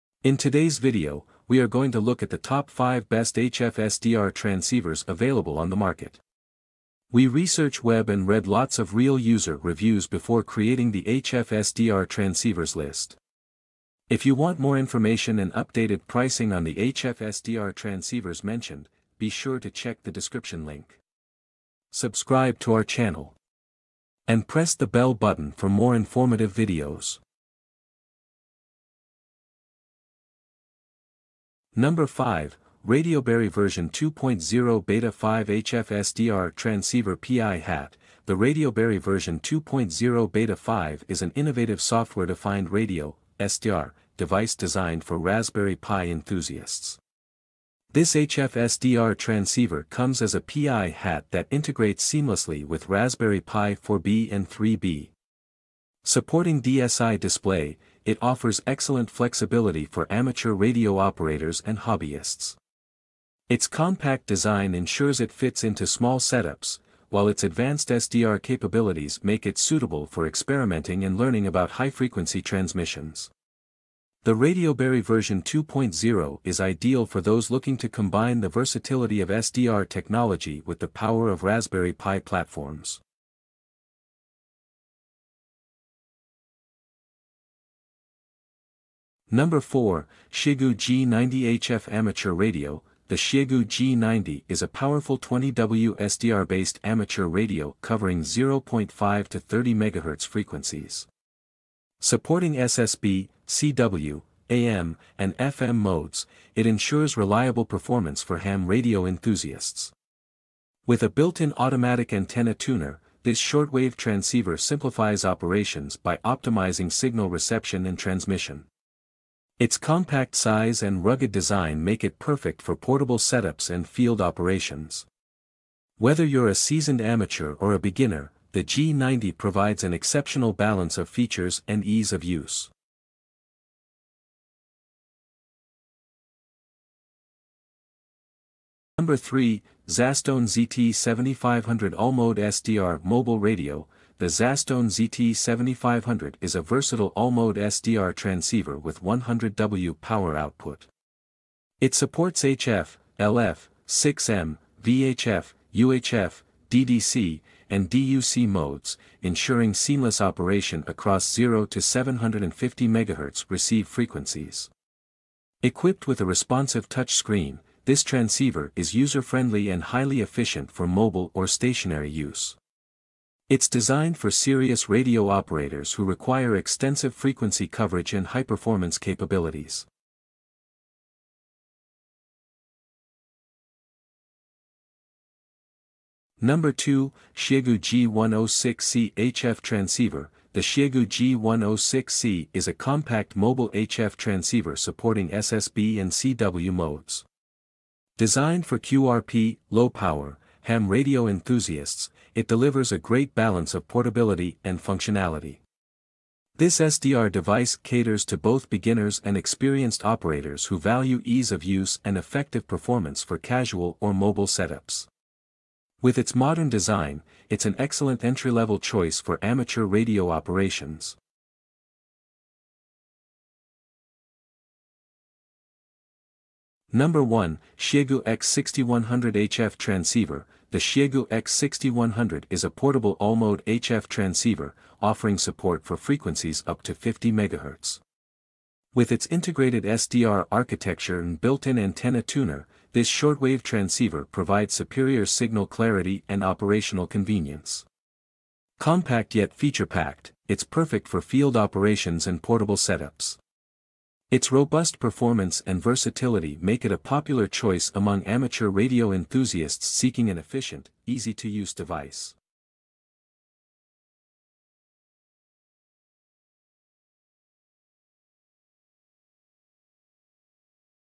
Best Transceiver: Top 5 Best HF SDR Transceivers In 2025
Today's video, I will explain to you the TOP 5 Best HF SDR Transceivers Guides & Review In 2025. I think you will be satisfied to see the review list of our HF SDR Transceivers.

.🙂 HF SDR Transceivers From Aliexpress 🙂.

.🙂 Full Details 🙂.
5. Radioberry V2.0 Beta5 HF SDR Transceiver PI HAT

4. Xiegu G90 HF Amateur Radio

3. ZASTONE ZT7500 All-Mode SDR Mobile Radio

2. Xiegu G106C HF Transceiver

1. Xiegu X6100 HF Transceiver

Links: This video contains source links of products for full details and more information.

.🙂 Chapters 🙂.
0:00 Intro
0:26 Number Five
1:24 Number Four
2:14 Number Three
3:01 Number Two
3:42 Number One
4:22 Conclusion